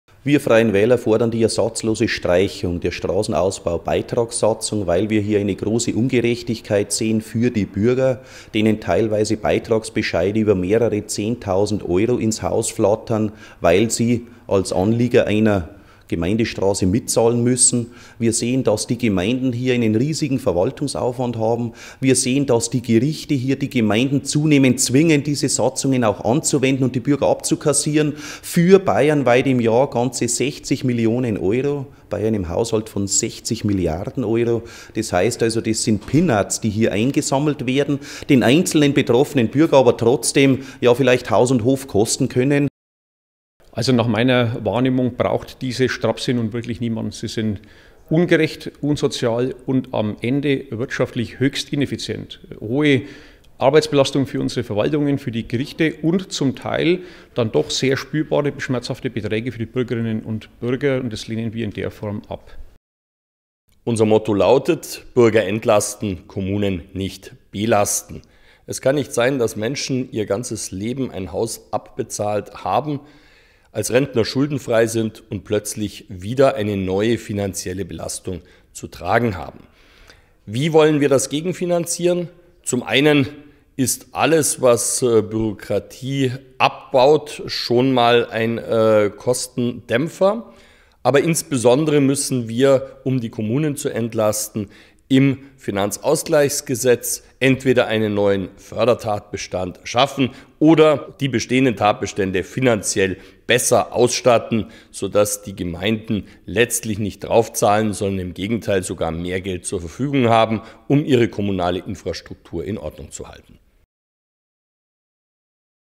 FREIE WÄHLER fordern Aus für Straßenausbaubeiträge in Bayern
Die FREIE WÄHLER Landtagsfraktion fordert die Abschaffung der Straßenausbaubeiträge in Bayern. Das erklärten Fraktionschef Hubert Aiwanger, sein Fraktionskollege Bernhard Pohl sowie der Landrat des Landkreises Nürnberg-Land, Armin Kroder, bei einer Pressekonferenz im Bayerischen Landtag. Aiwanger: „Der Ärger, den diese Beiträge auslösen, ist deutlich größer als der Nutzen. Das System ist ungerecht, streitanfällig und verursacht in den Kommunen erheblichen Verwaltungsaufwand sowie politischen Ärger. Abschaffen – wie in Baden-Württemberg, Berlin oder München – ist die konsequente Lösung.“